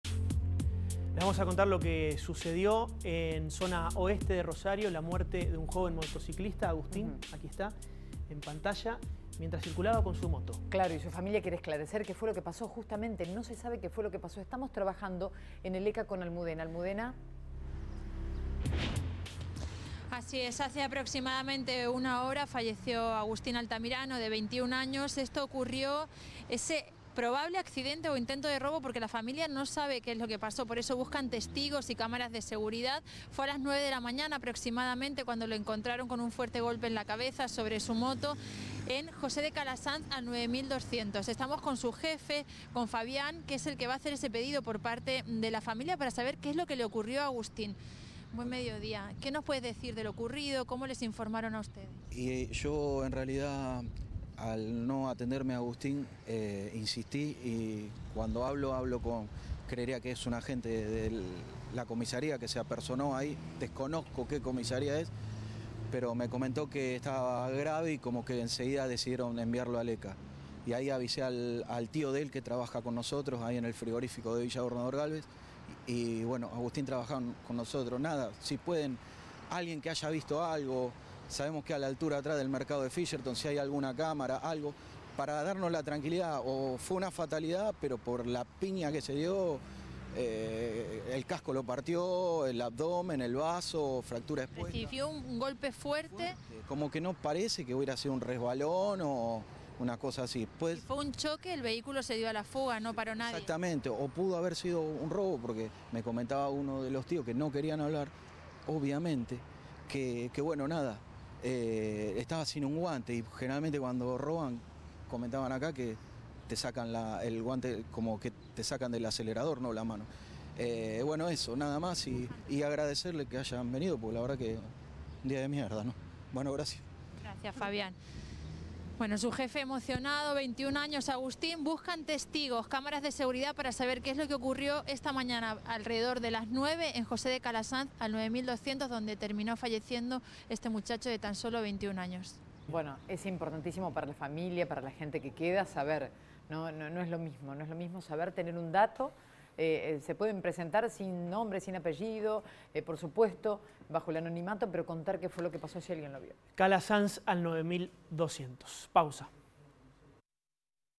Piden testigos sobre un hecho fatal
La familia desconoce si fue un robo o un siniestro vial ocurrido en Calasanz al 9200